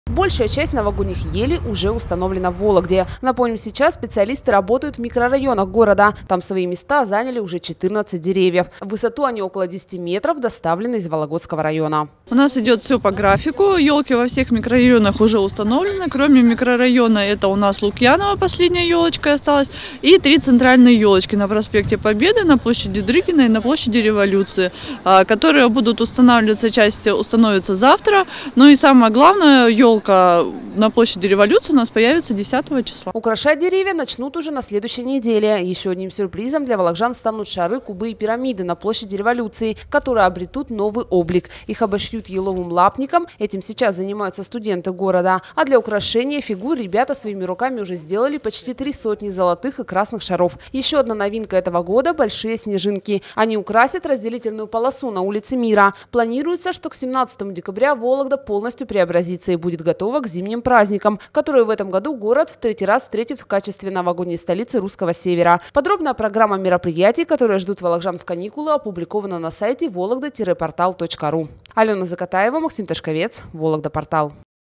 Большая часть новогодних елей уже установлена в Вологде
Сейчас специалисты работают в микрорайонах областной столицы. В разных точках города установлено 14 деревьев. В высоту они около 10 метров. Доставлены ели из Вологодского района.